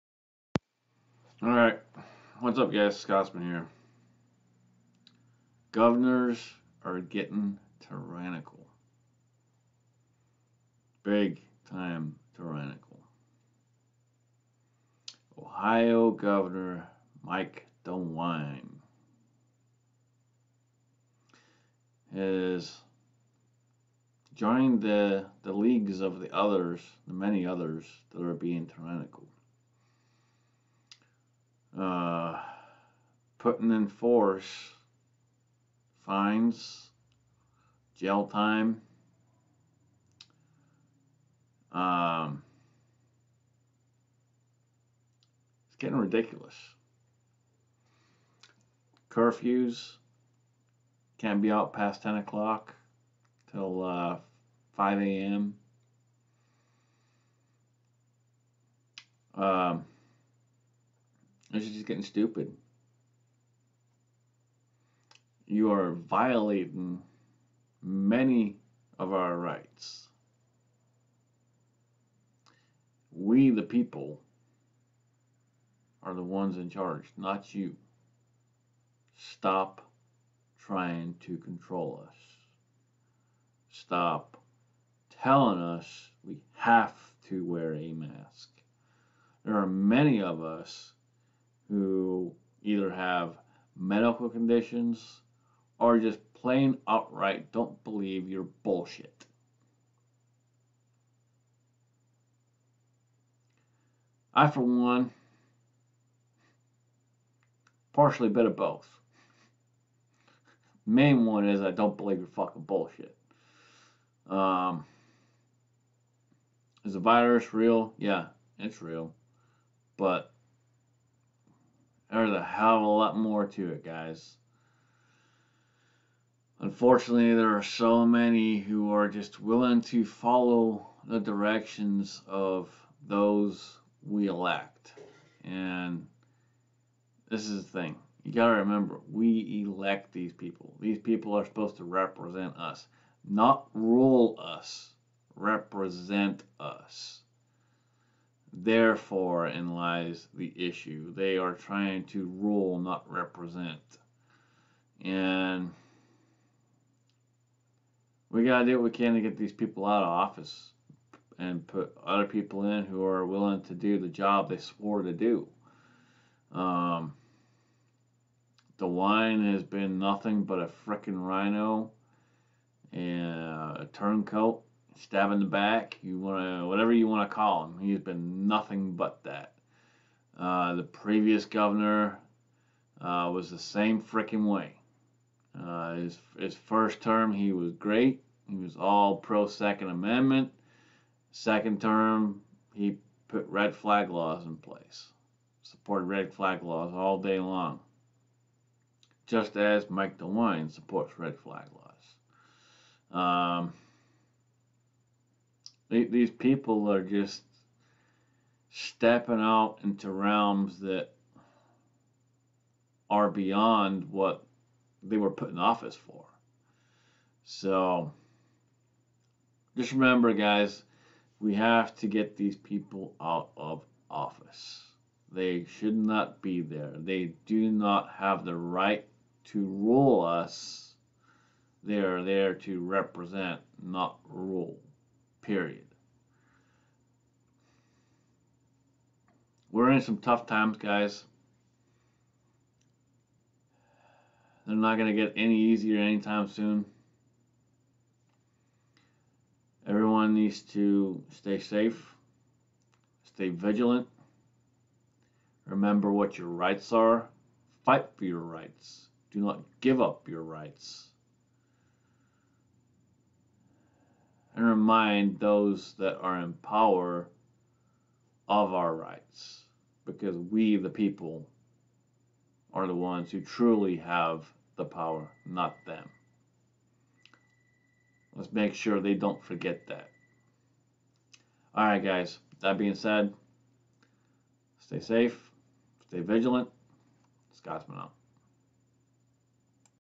Tyrannical Governors
Keep up the fight they think we are weak, they believe we will follow their unconstitutional BS

This Virus has a 99% survival rate, somehow magically they have a vaccine that is 90% effective when after over 60 years of the regular flu the vaccine is only 40% effective wake up people it's nothing but control

We must remind them they are not in charge